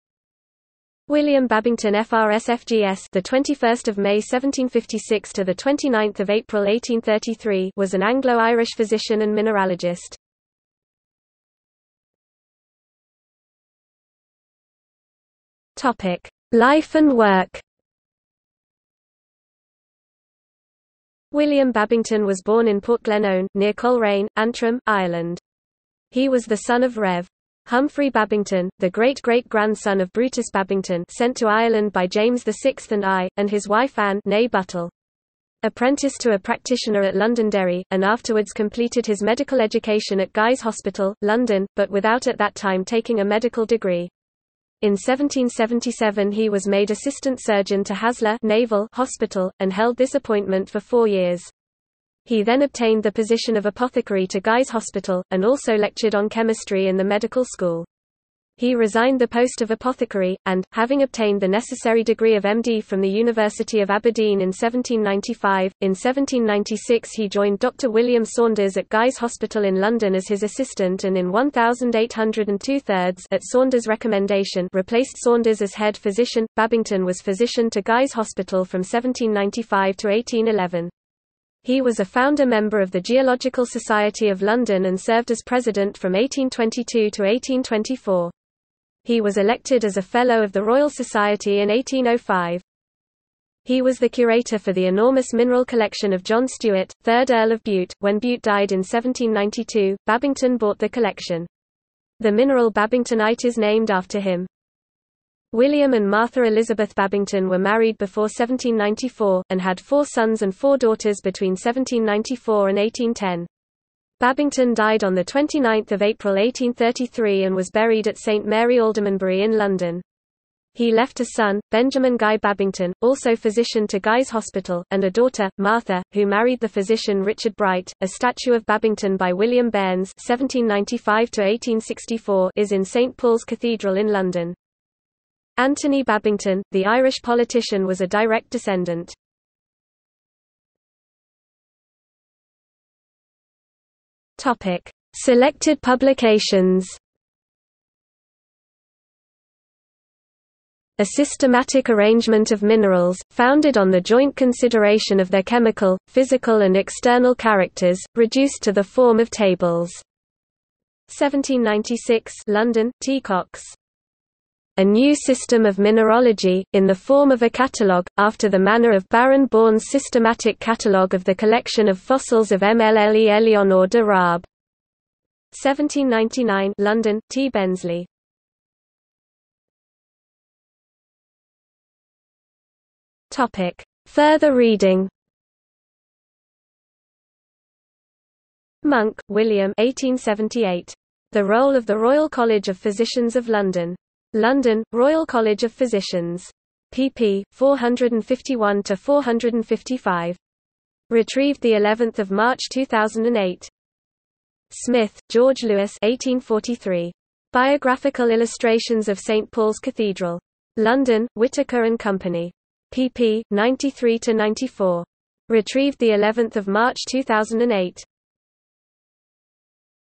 William Babington (physician) | Wikipedia audio article
00:00:12 1 Life and work
00:02:20 2 Selected publications
00:02:59 3 Further reading

- Socrates

SUMMARY
William Babington FRS FGS (21 May 1756 – 29 April 1833) was an Anglo-Irish physician and mineralogist.